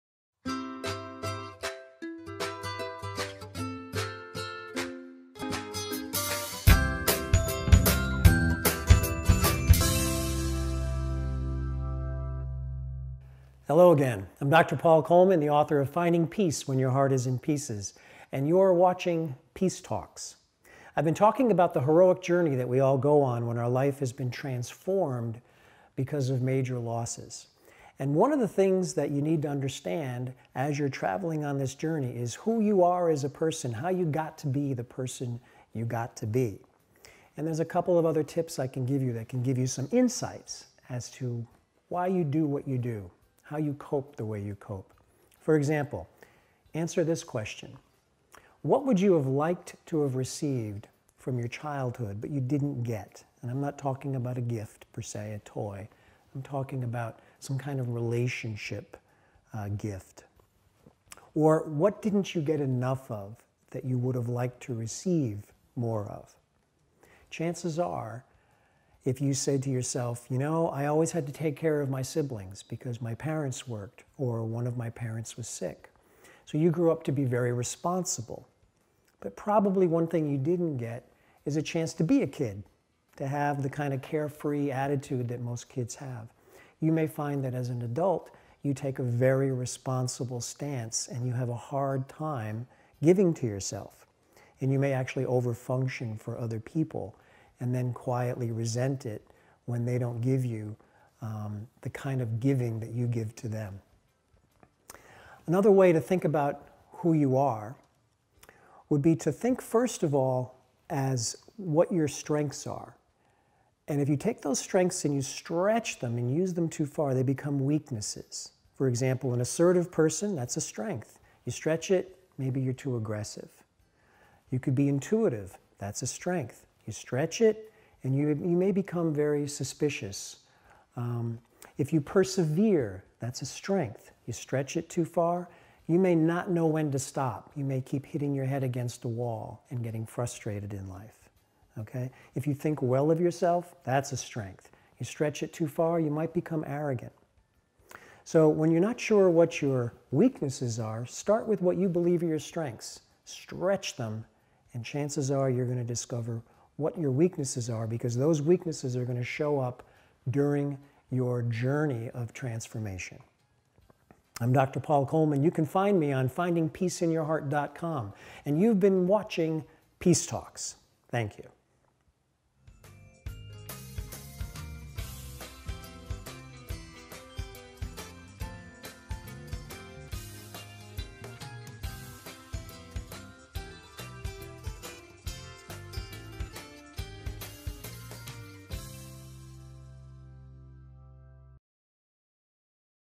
KnowingYourself2
Self-knowledge is a key factor in personal growth and transformation. On your journey it is important to discover NEW strengths--and that usually happens only when being forced to deal with a situation that is highly challenging. But in the process you will also become aware of areas of weakness--areas that must be released and overcome if you wish to arrive at a new, meaningful destination in your life's journey.